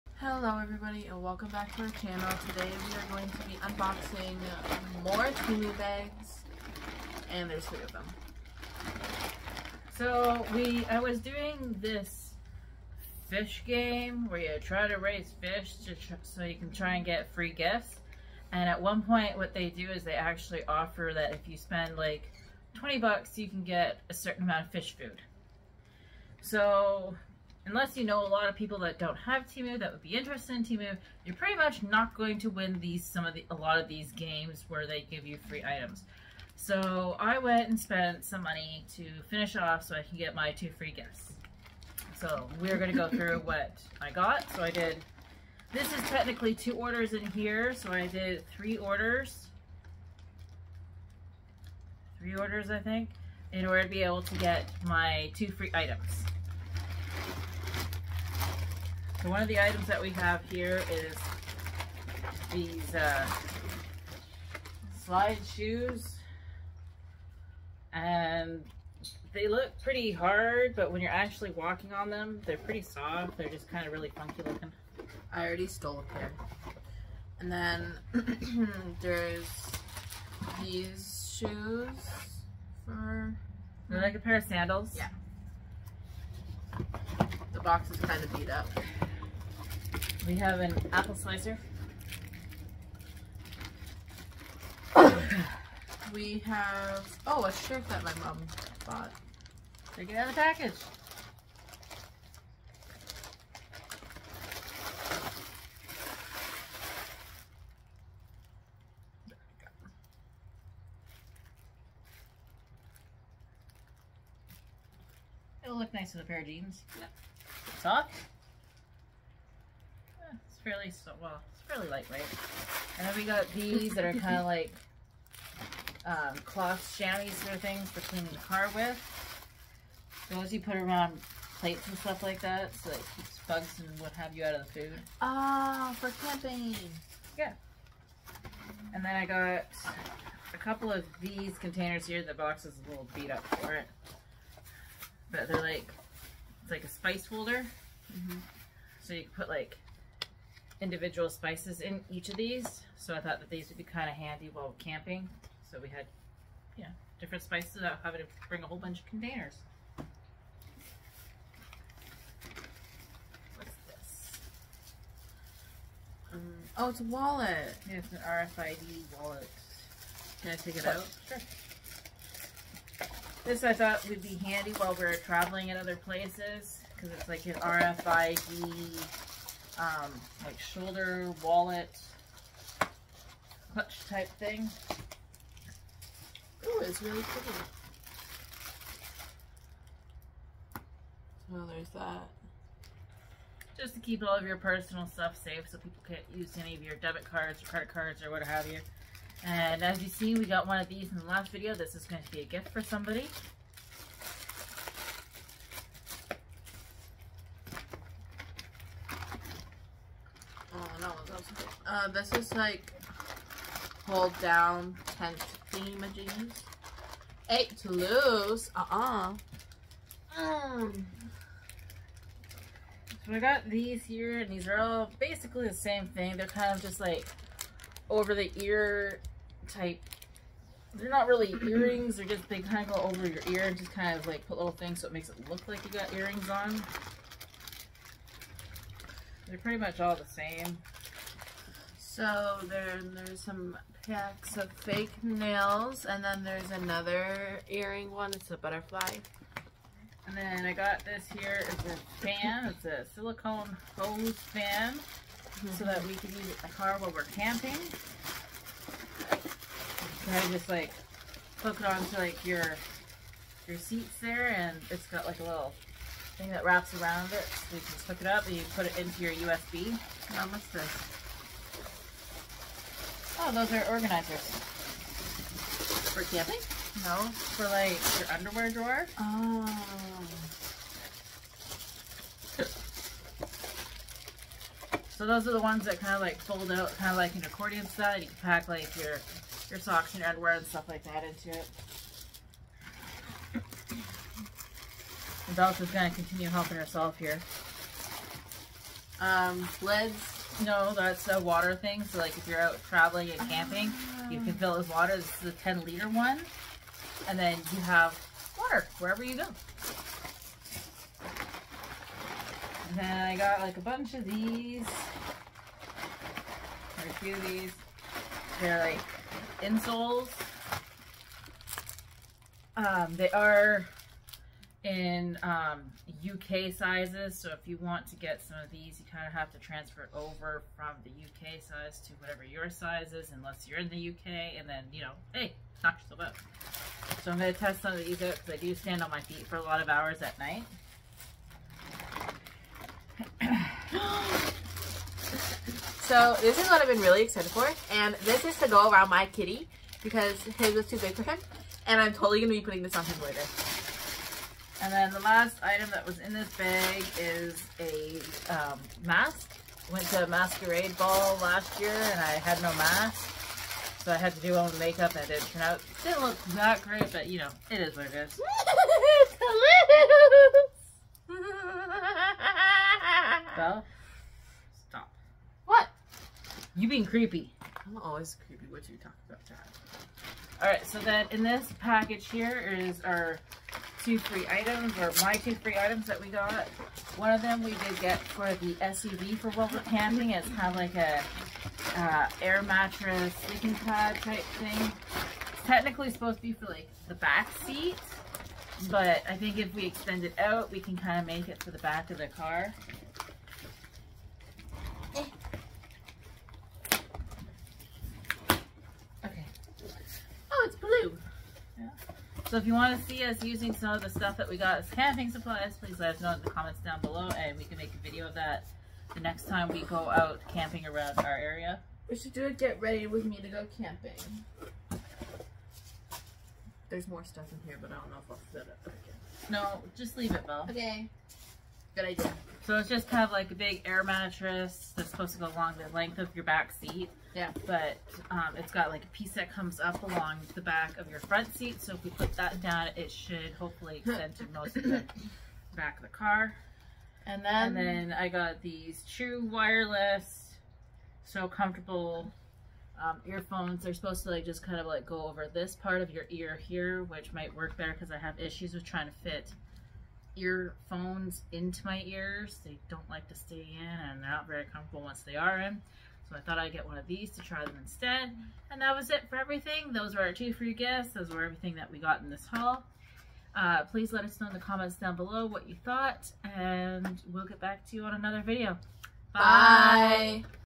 Temu haul #2 plus my free gifts!
We are opening another temu haul plus a couple of free gifts.